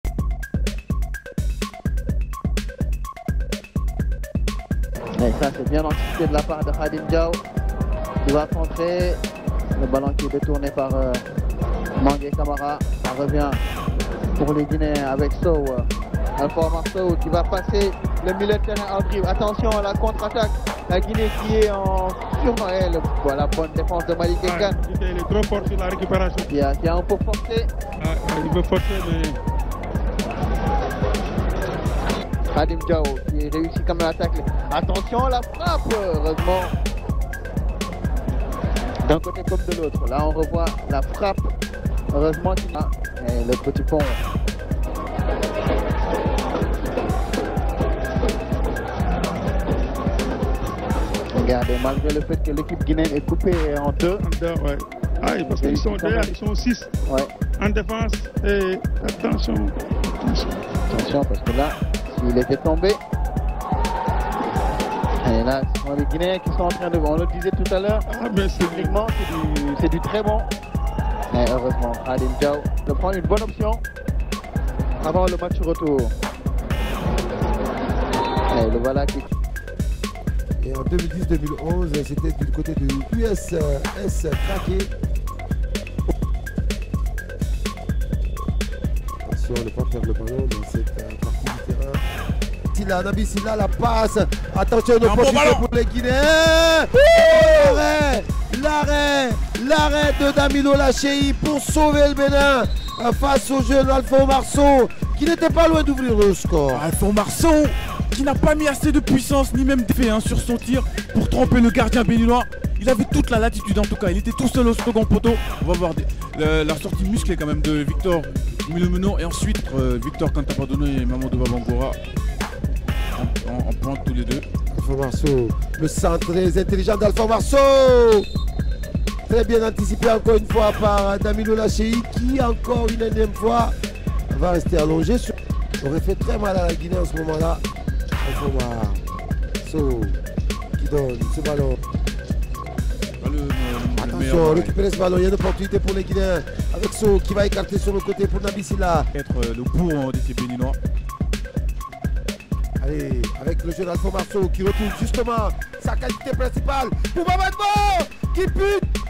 Alpha Oumar Sow (UFOA 2019)
Milieu Offensif et Excentré International A' Guinéen. Vainqueur du trophée de Consolation du Tournoi UFOA 2019 (SÉNÉGAL) et demi finaliste du CAN 2021 (CAMEROUN)